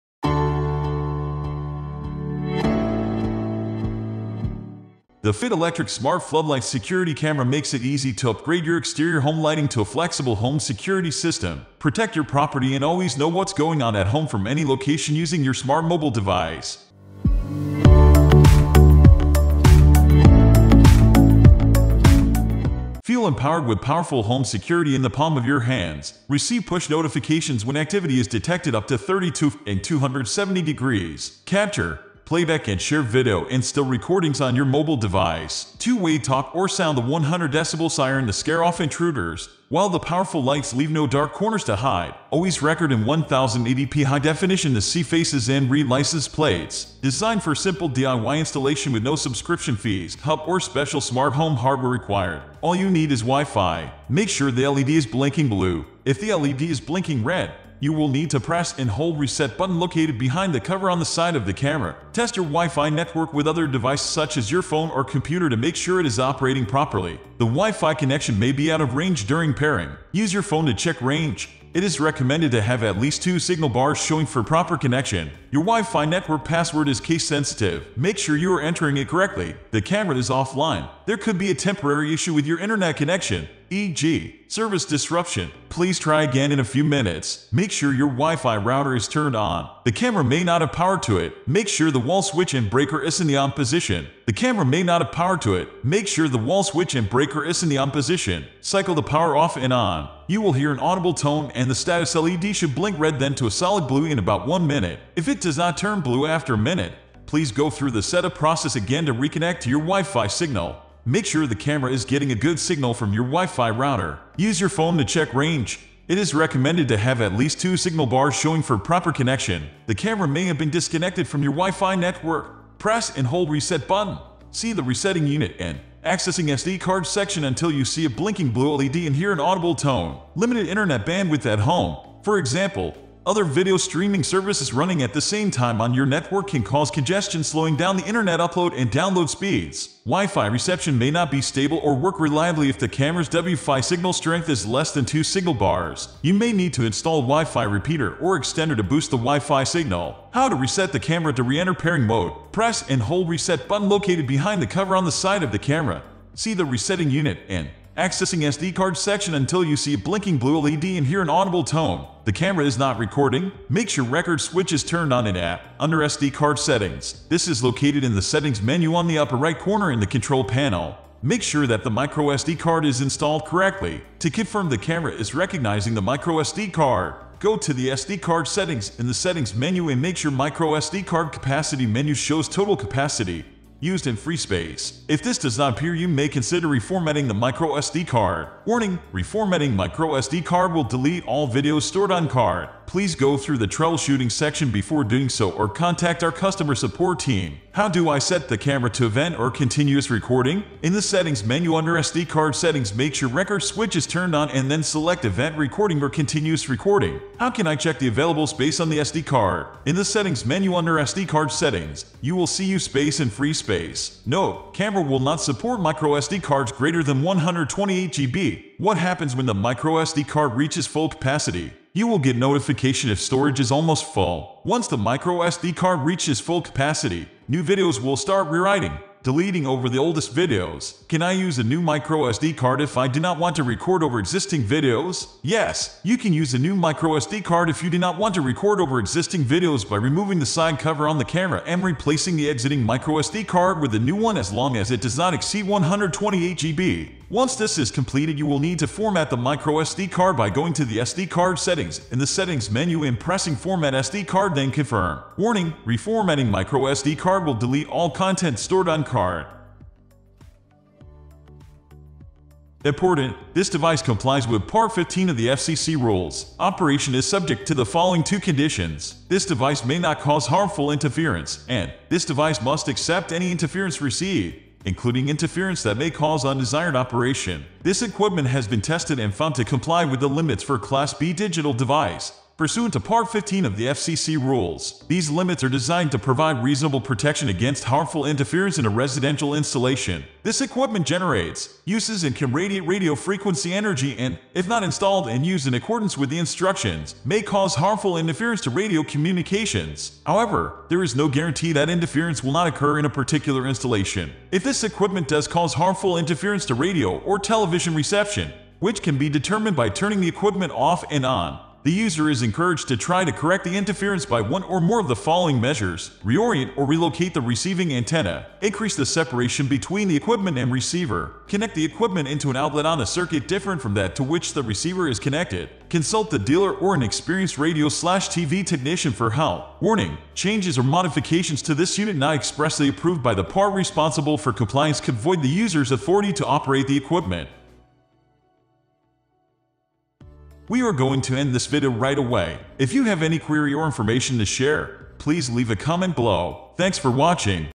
Feit Electric LED Dual Head Motion Sensor Security Light with Camera - Should You Buy? (2024)
The Feit Electric Smart Floodlight Security Camera makes it easy to upgrade your exterior home lighting to a flexible home security system. Protect your property and always know what’s going on at home from any location using your smart mobile device. Feel empowered with powerful home security in the palm of your hands.